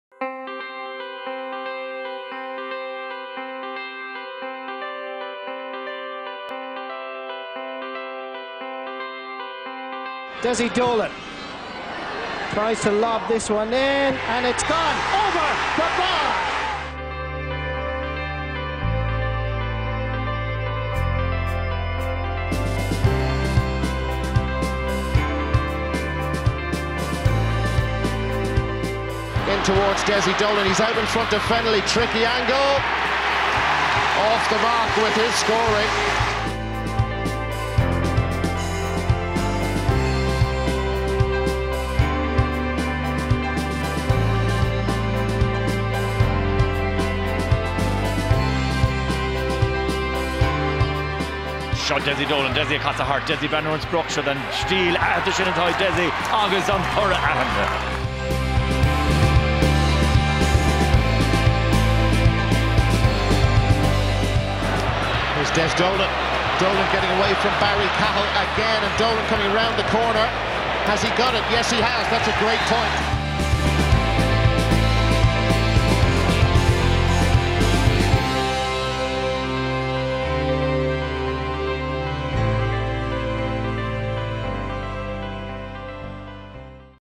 Leinster GAA welcome Westmeath football hero Dessie Dolan into our Hall of Fame
Garrycastle genius Dessie Dolan, a star forward on the first ever Westmeath team to win the Leinster SFC title and lift the bDelaney Cup, is the newest member of our Hall of Fame.